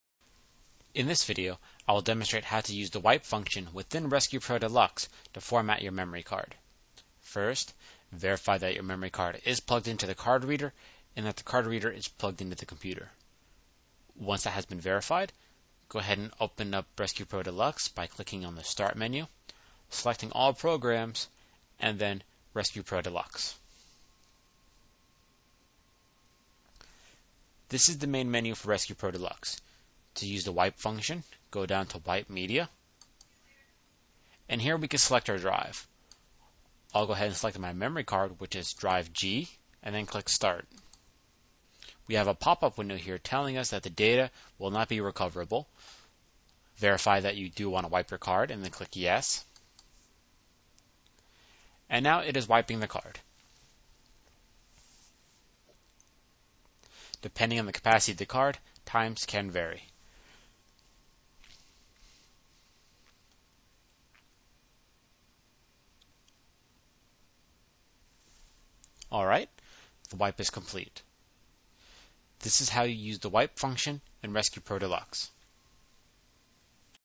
RescuePro Deluxe Wipe
This video covers the file wipe function of RescuePRO® Deluxe.
RescuePRO® Deluxe is a file and data recovery product for recovering deleted files from all types of digital media. It recovers a variety of file types including images, video, documents, music and hundreds of other popular file types.